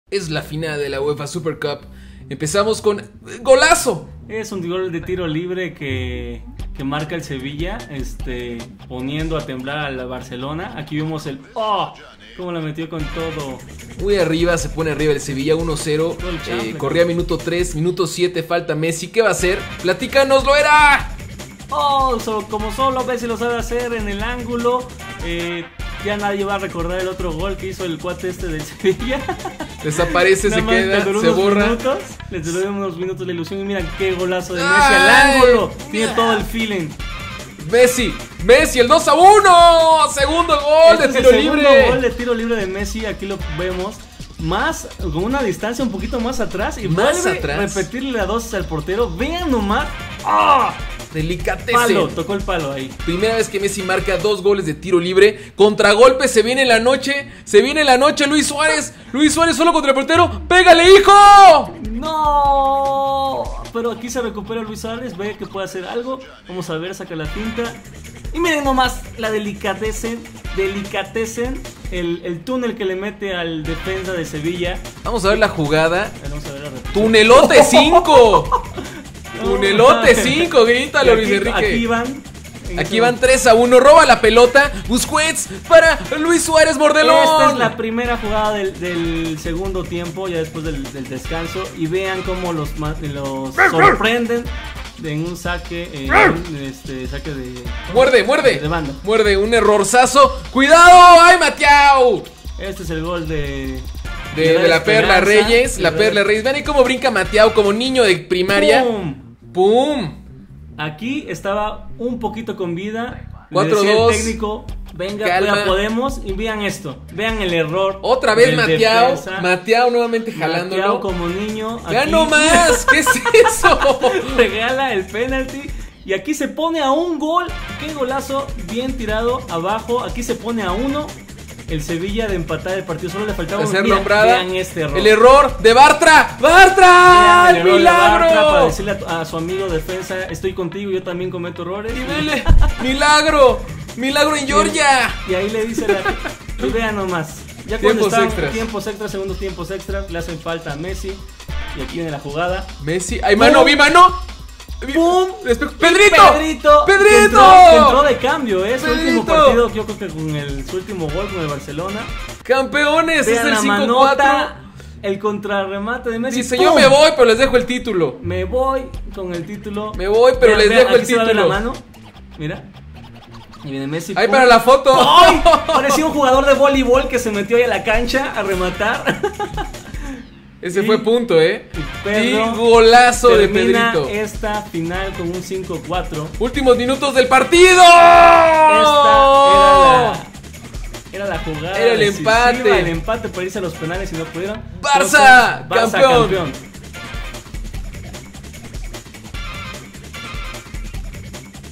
BARCELONA VS SEVILLA UEFA SUPER CUP 2015 11/AGO/2015 FIFASTICOS
Barcelona vs Sevilla por la UEFA SUPER CUP 2015 TODOS LOS GOLES y narración de fifasticos.